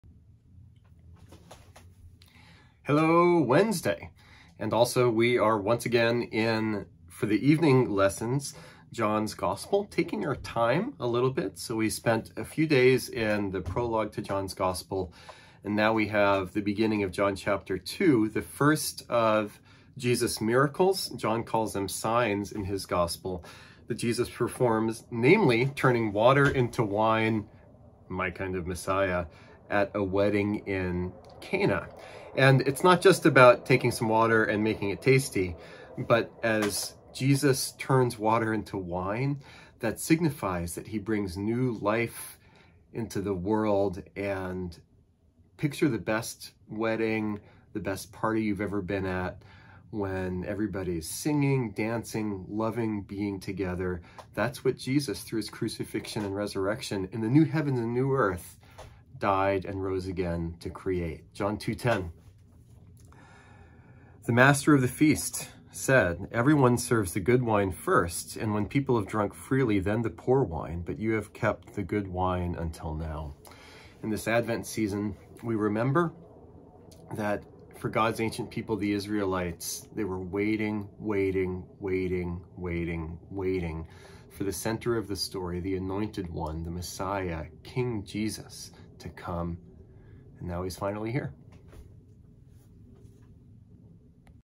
Advent Devotional Supplement - Day Three
Rev. Jim Angehr delves deeper into John 2 during Advent for Wednesday's evening lesson.